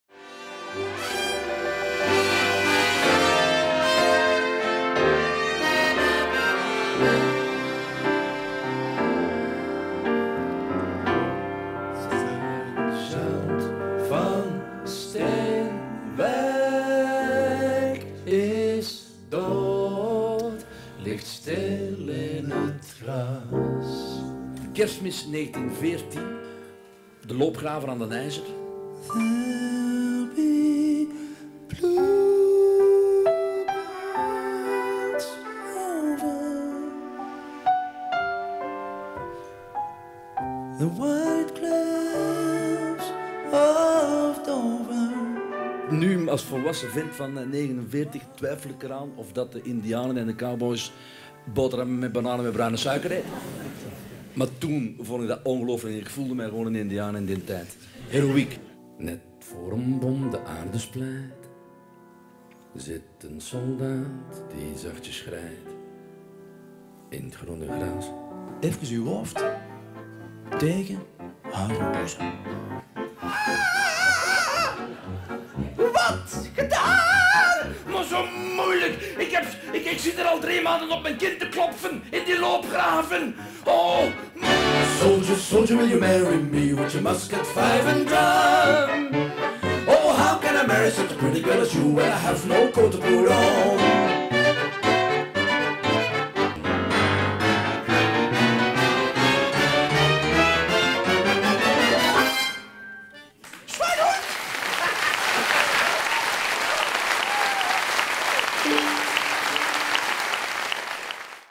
HET BESTAND Kommil Foo (trailer)
Samen met de 6 beste muzikanten van de wereld zingen Mich en Raf Walschaerts de meest pakkende oorlogsliederen uit het wereld-repertoire, vertellen ze ontroerende, hilarische verhalen in hun eigen onnavolgbare stijl. En oorlogsliederen mag u breed opvatten: van heroïsche strijd-liederen (oorlog op het slagveld) tot de meest verscheurende liefdes-songs (oorlog in het hart).

Omdat oorlog niet alleen de pijn is, de verwoesting, het verdriet. Het is evenzeer de bloem op de mesthoop, de liefde, de slapstick, de humor.

Stof genoeg voor Kommil Foo om een voorstelling te maken over hun eigen, hoogst-persoonlijke strijd. Gezongen wordt er in het Nederlands, Frans en Engels. Verteld en gespeeld op leven en dood: hilarisch en tragisch tegelijk, zoals het leven zelf.

Met Raf Walschaerts: gitaar, zang
Mich Walschaerts: viool, zang
Gwen Cresens: accordeon, bandoneon
Bart Van Caenegem: piano
Nico Schepers: trompet
Carlo Nardozza: trompet en bugel
Carlo Mertens: trombone
Berlinde Deman: bas-tuba

Productie: Caroline lanoye
Techniek: Koen Bellens